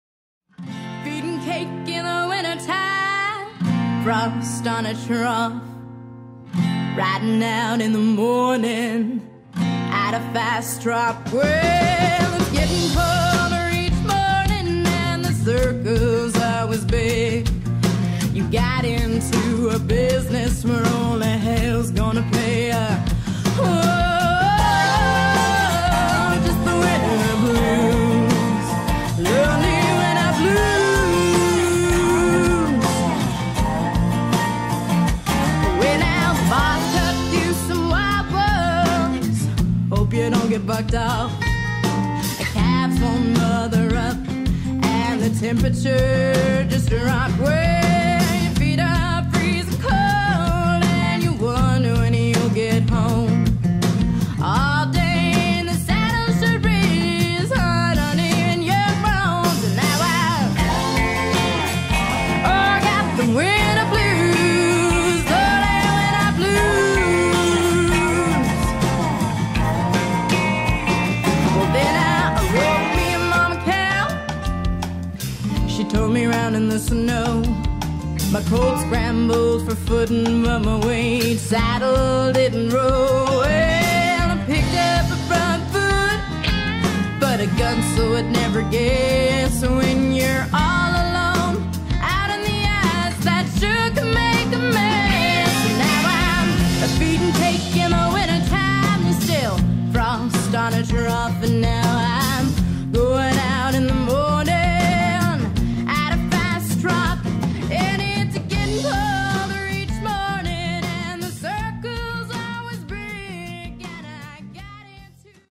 SOLD: Steely Blue Night
Steely Blue Night, Blue, is a beautiful well-minded horse for sale in Lewistown, MT.